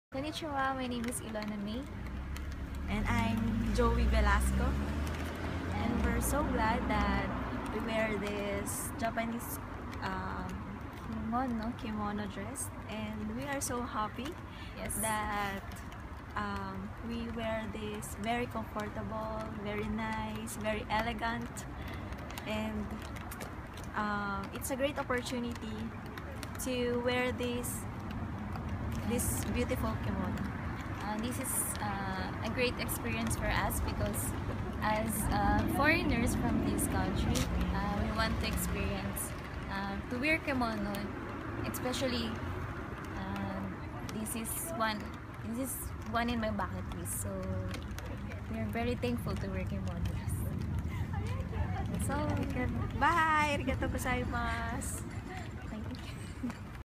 KIMONO Experience - Megane Bridge in Nagasaki
Our guests from the Philippines | Kimono Rental Café Fumiyaen